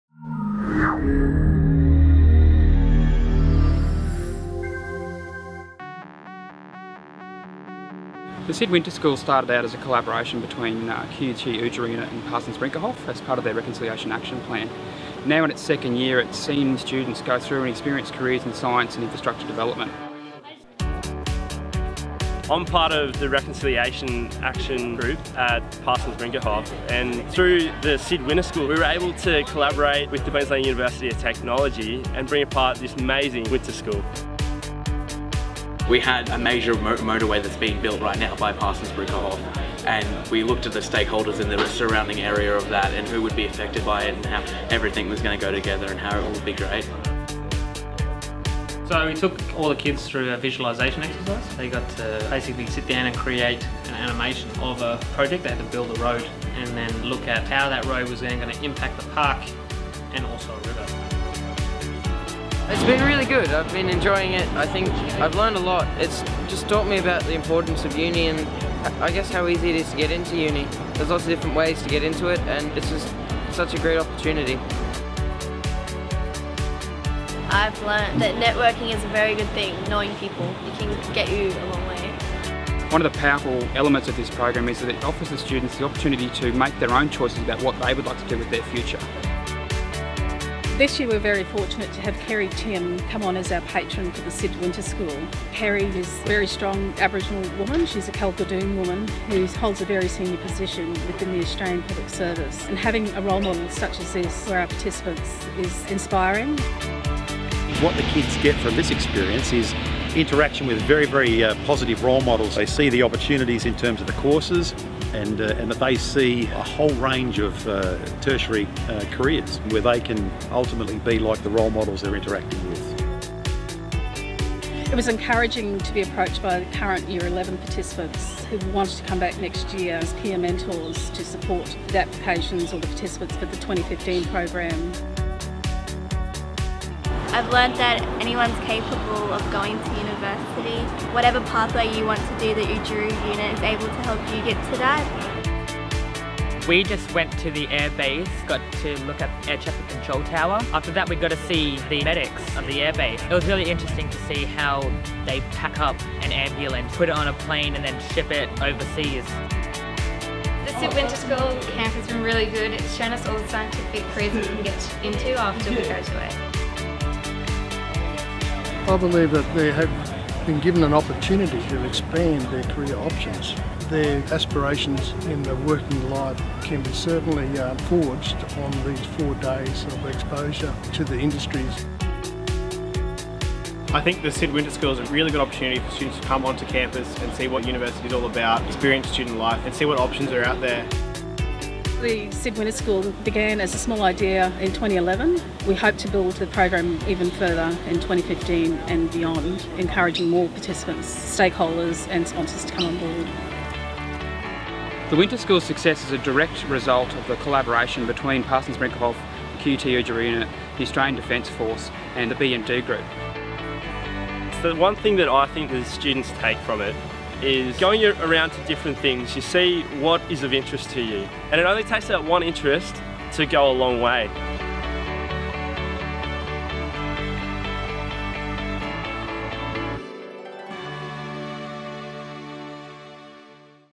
Indigenous Australian Science and Infrastructure Development (SID) Winter School
The Indigenous Australian Science and Infrastructure Development (SID) Winter School is a joint initiative between the Oodgeroo Unit, Queensland University of Technology (QUT) and Parsons Brinckerhoff (Australia-Pacific) – led by the Brisbane Regional Office. The SID Winter School aims to increase access and participation in higher education for Aboriginal and Torres Strait Islander students. The SID Winter School is designed to inform students about a variety of options to consider with information on courses and careers.